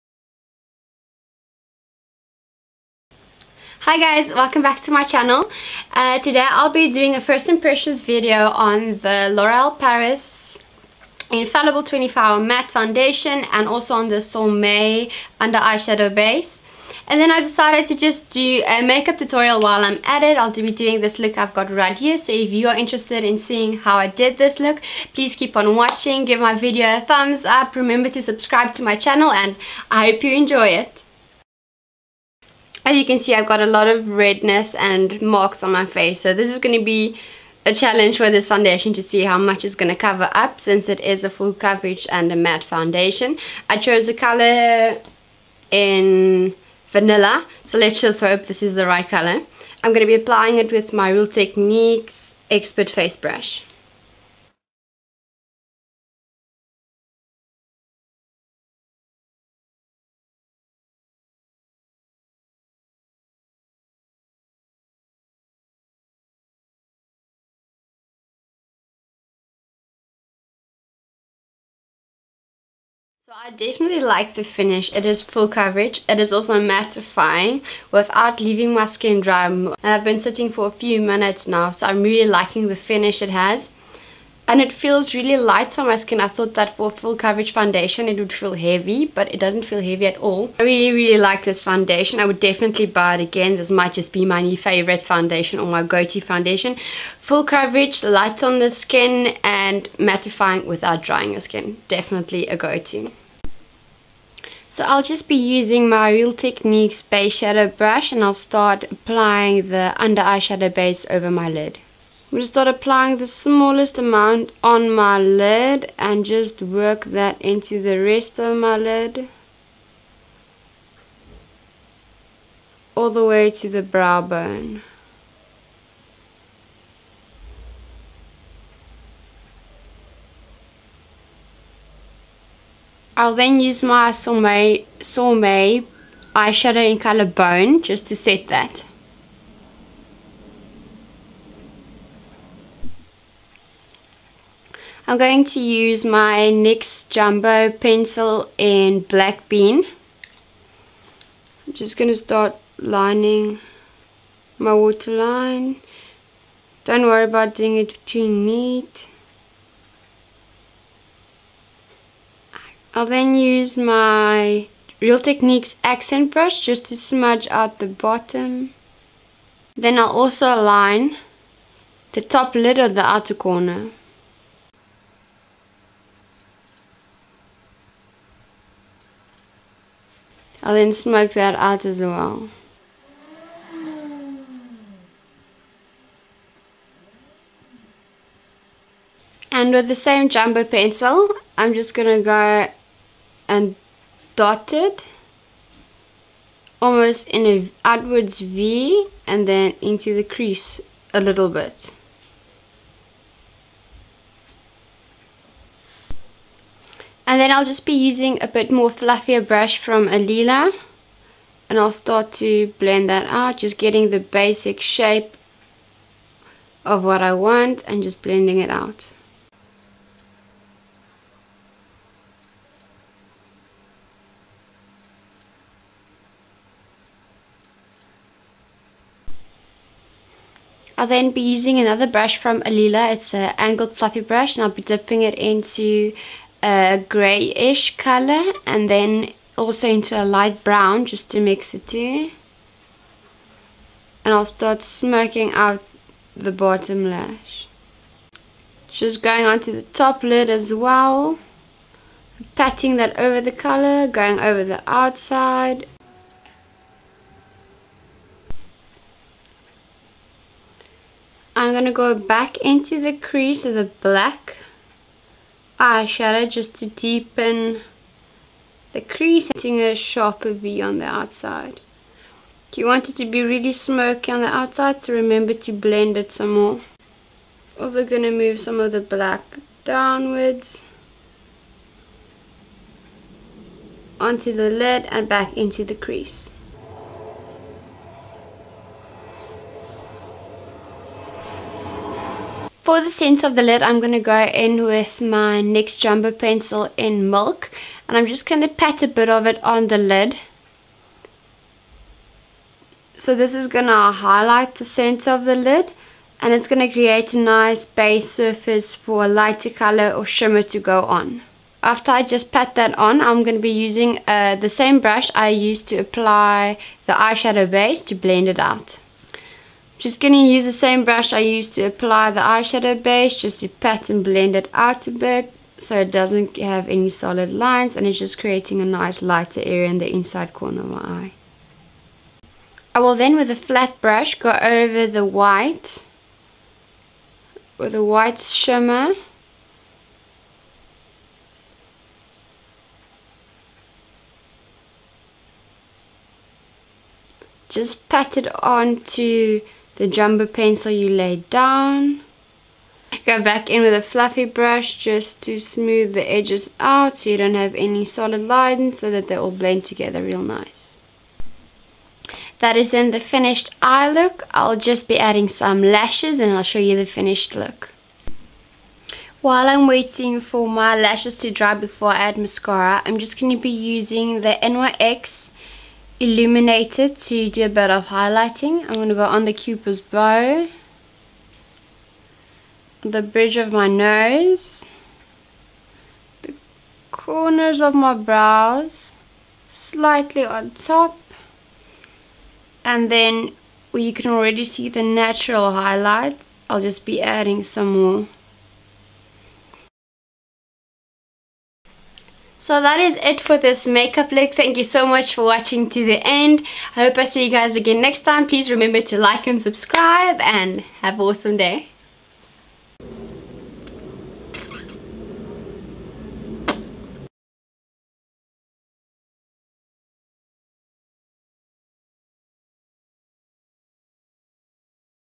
First Imp on L'OREAL INFALLIBLE 24H-MATTE // Makeup tutorial
Hallo everyone !
I really liked doing this first impression on the LÓREAL INFALLIBLE 24H-MATTE FOUNDATION ! Love the product, would buy it again. I liked that even though it is a matte and full coverage foundation, it never dried out my skin throughout the day. LIKE2LOVE

Then the Sorme under shadow base(drooool). A very important product for long lasting eyeshadow that does not crease or sweat off during the day.

Other makeup products:

Sorme eyeshadow - Bone
NYX Jumbo pencils - Black Bean
                                    - Milk
Inglot eyeshadows - Black
                                   - Grey/Brown
                                   - Shimmer white
                                   - Brick orange
LASHES - EYELURE 116
Sorme : Lip liner - Pure Rose
               Lipstick - Feminine
NYX : MATTE BRONZER
           ILLUMINATOR

Love, Melissani